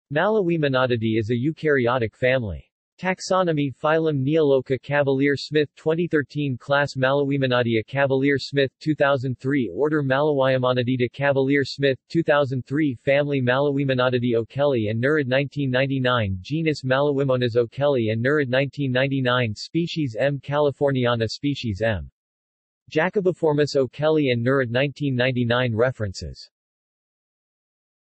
Neolouka | Wikipedia audio article
This is an audio version of the Wikipedia Article:
Neolouka

SUMMARY
Malawimonadidae is a eukaryotic family.